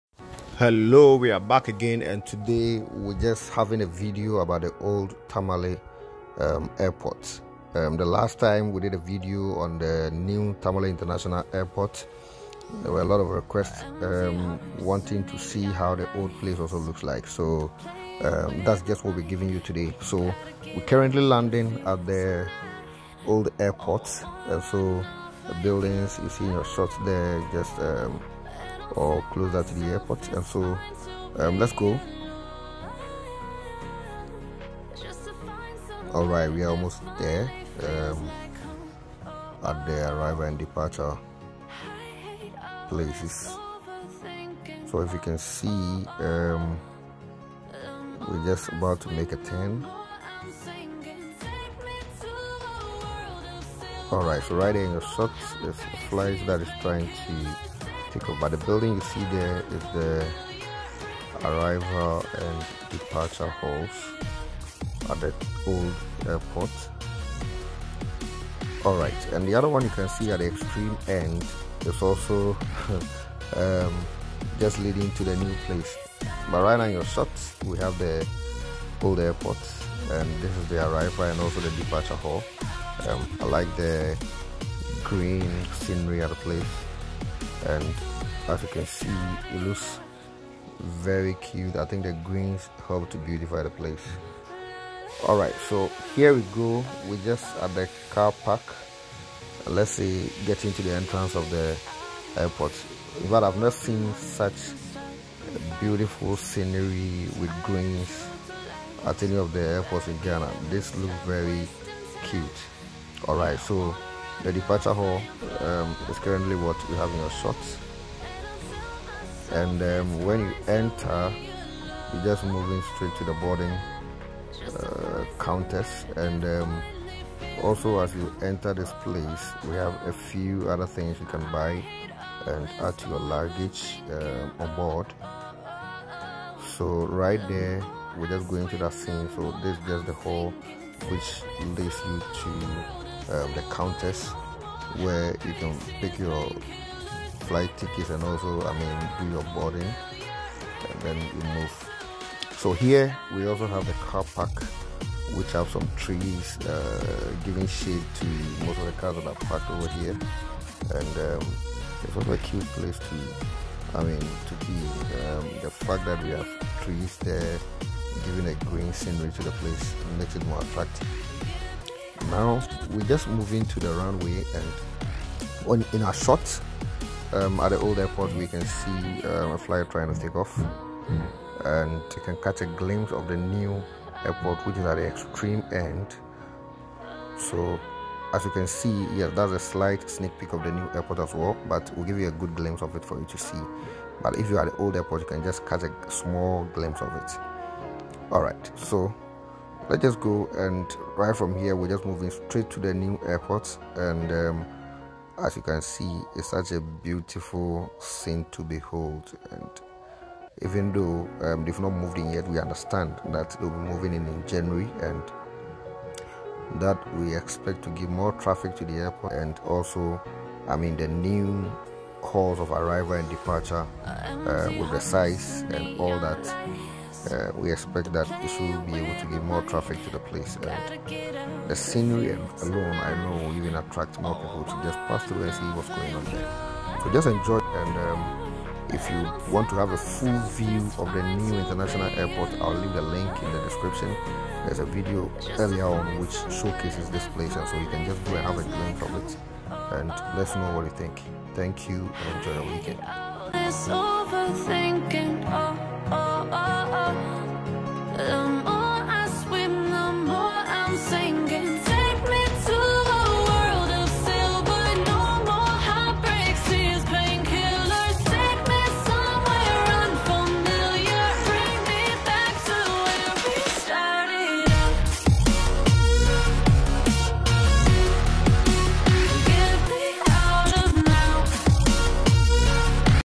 The Old Tamale International Airport
Today we are taking a quick tour of the old Tamale Airport with a sneak peak of the New Tamale International Airport. Let's go